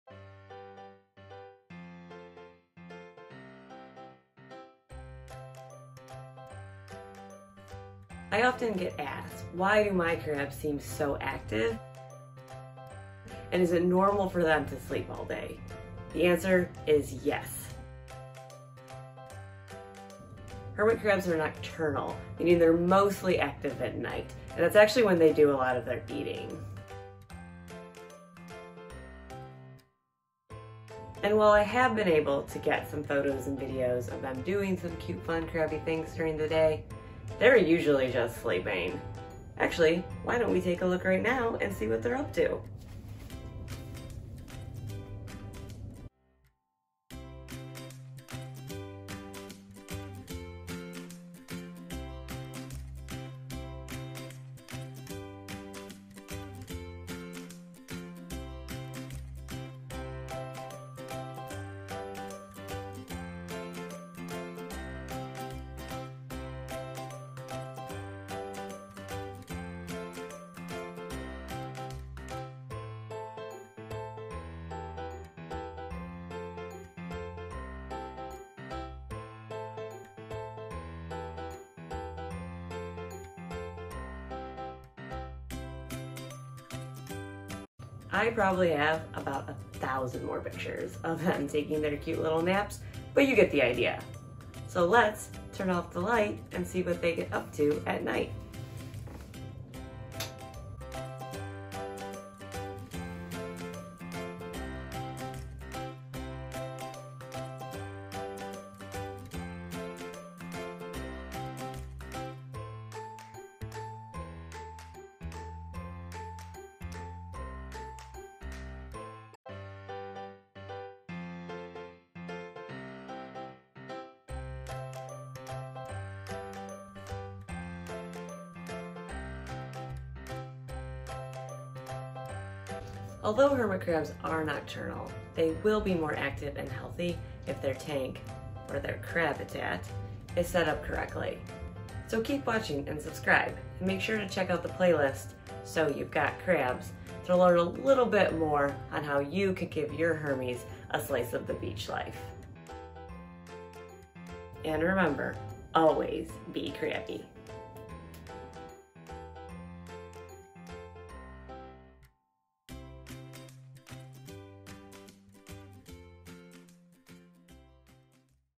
Are hermit crabs nocturnal? And is it normal for them to sleep all day???
I often get asked why my hermit crabs seem so active. Although I have gotten some great footage of them doing cute crabby things, normally during the day they are just chilling out or asleep. Hermit crabs are nocturnal and they are much more active at night. They eat, run around and climb all over the place until the sun comes up, then it's back to sleeping on the beach all day.

But, hermit crabs are more likely to be somewhat active during the day if their tank (or "crabitat") is set up properly. Hermit crabs need high humidity and a damp, warm environment to breathe, fresh and salt water pools, plenty of fruit, vegetables, fat and protein to eat, along with deep substrate for molting. They actually require quite a bit of care, but have no fear! I'm here to help you set up your hermit crab tank so they can live long, healthy, happy and crabby little lives.

- thanks!
And love my new Wildlife Brewing t-shirt from Idaho!